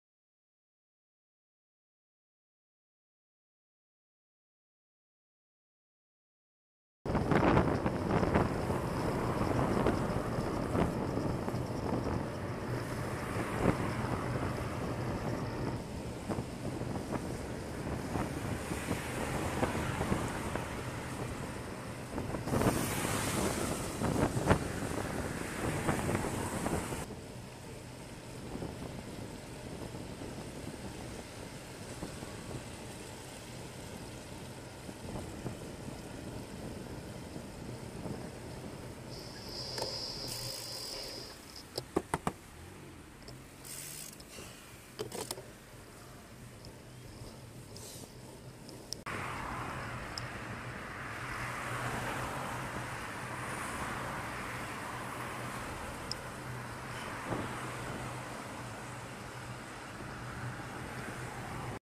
Dangerous Close passes Syston to Oadby
A selection of close passes on a ride from Syston to Oadby through Leicester.
I had very bright flashing lights front and rear (latest Cycliq on high power). I was wearing a very bright blue jacket with reflective details and cycling trousers with high vis elements.
The whole ride is available to the Police if needed.